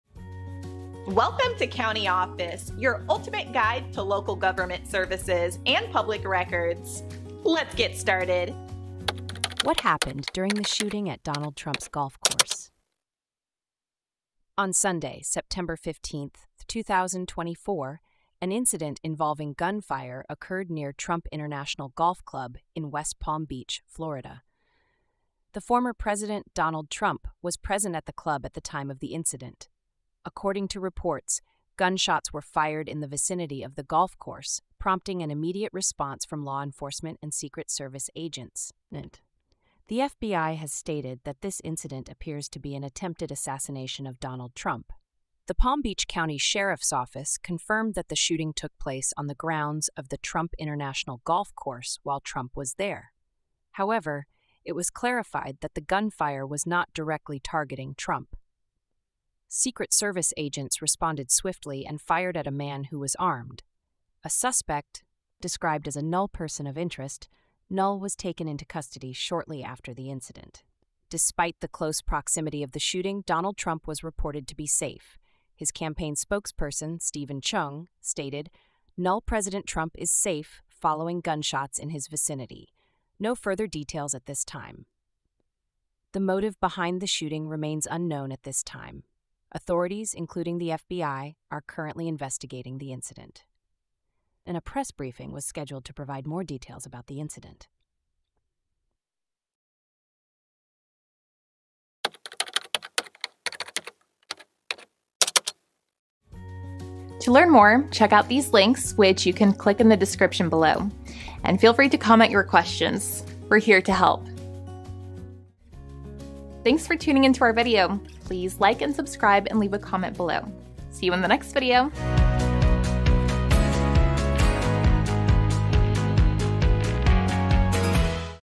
What Happened During the Shooting at Donald Trump's Golf Course? | CountyOffice.org
What Happened During the Shooting at Donald Trump's Golf Course? In this video, we delve into a recent incident that occurred near Trump International Golf Club in West Palm Beach, Florida, involving the former President, Donald Trump. Gunshots were fired in the vicinity, leading to a swift response from law enforcement and Secret Service agents. The FBI has labeled this as an attempted assassination, though it was clarified that the gunfire was not directly targeting Trump. The suspect was swiftly taken into custody, and despite the close call, Trump was reported safe. The motive behind the shooting remains unclear as investigations are ongoing.